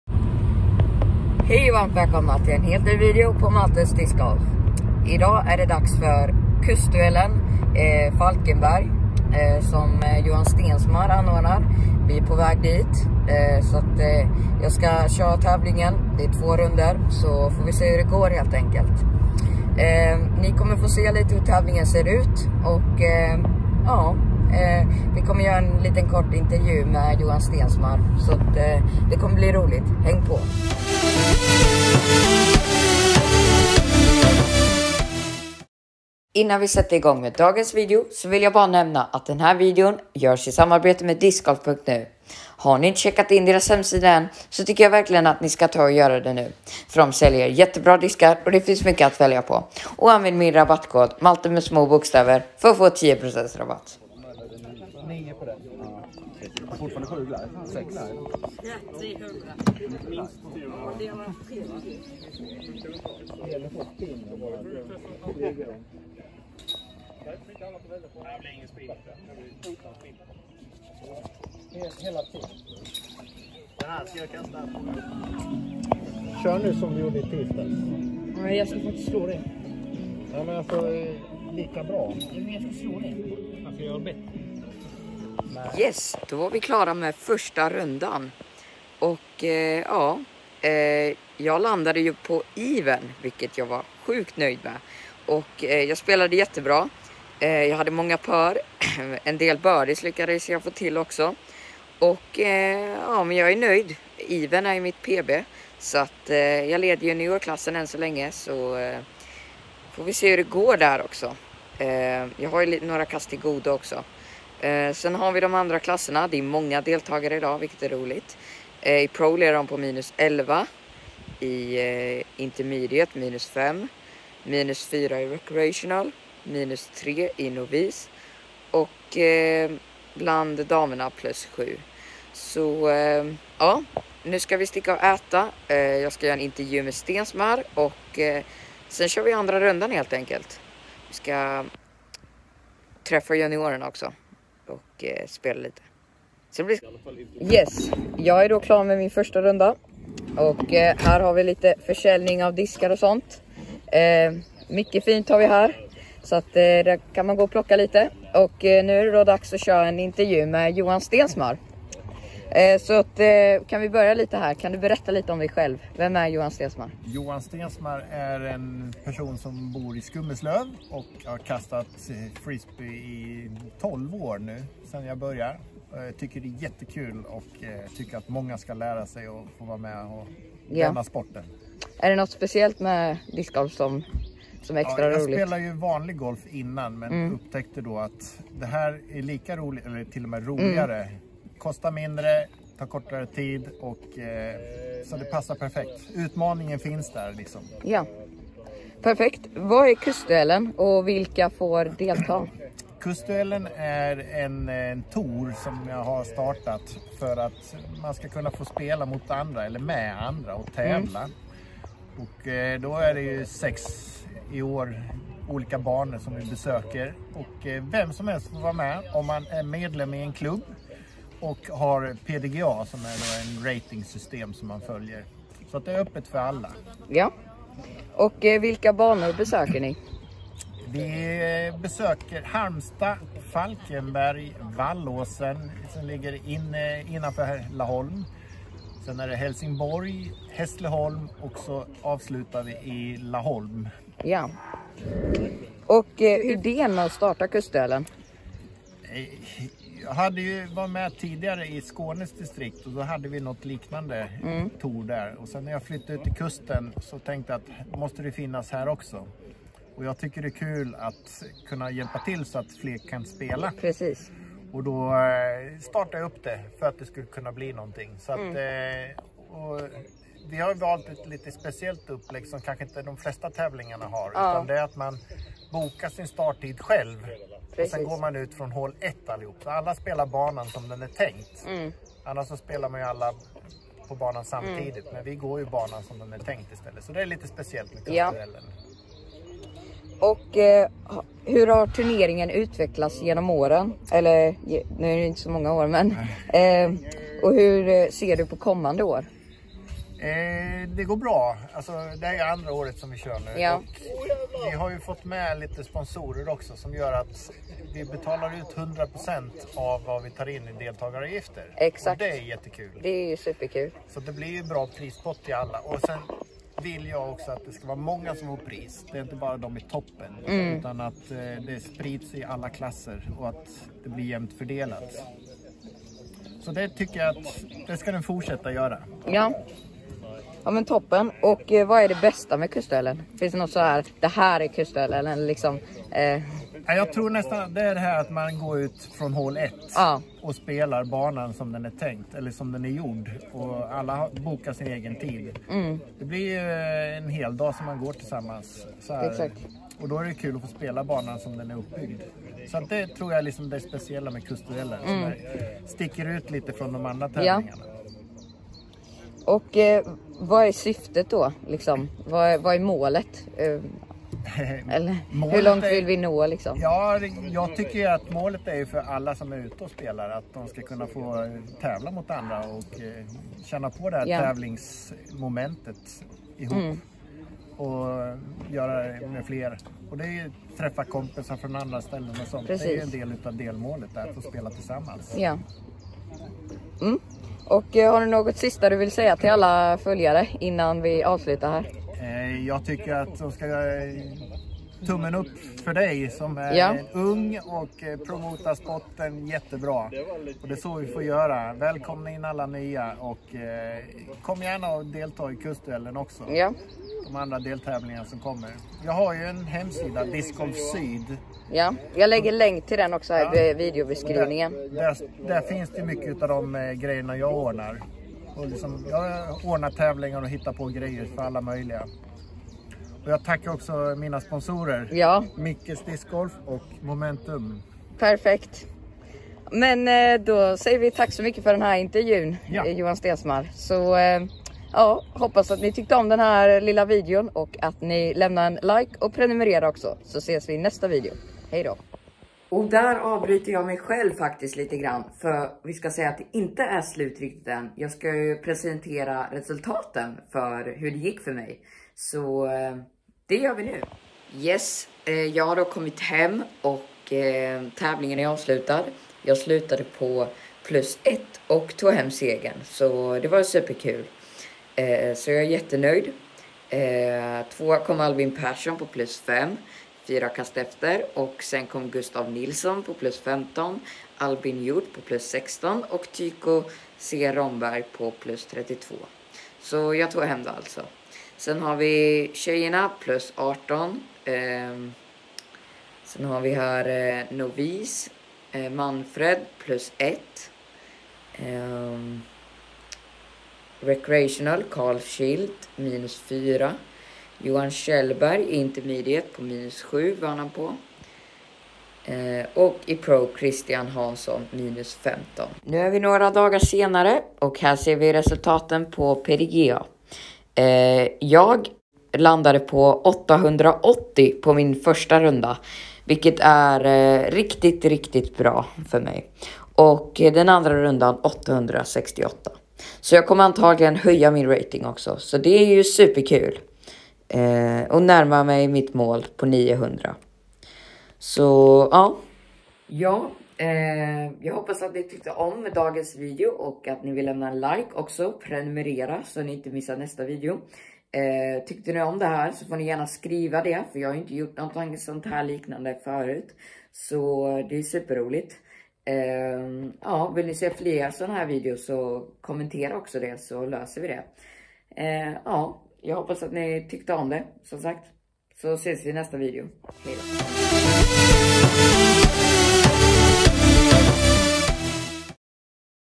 Kustduellen 2022- Falkenberg och Intervjuv med Johan Stensmar
Idag så tävlar jag i Kusduellen i Falkenberg och jag gör en liten blogg där. Under tiden får ni se hur det går för mig.

Dessutom så bjuder jag er på en härlig intevjuv med självaste TD Johan Stensmar där han svarar på bågen frågor gällande touren!

Håll till godo mina vänner och tack för ert stöd till kanalen 🤟